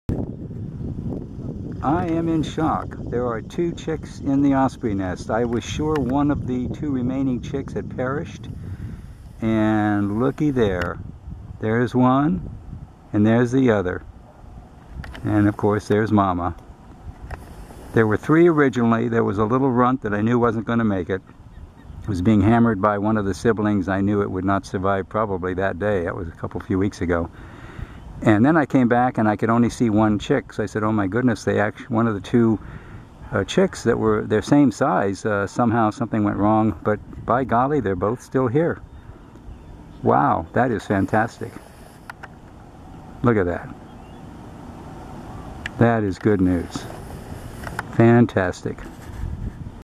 osprey nest followup - May 13, 2016
have been watching an osprey nest on the Hillsborough river in Tampa for weeks now.  Started out with three chicks.  One was very small and could not compete with the two larger siblings and it perished.  I visited the nest earlier this week and saw only one healthy chick and thought the other had been lost but it turns out they are both alive and well.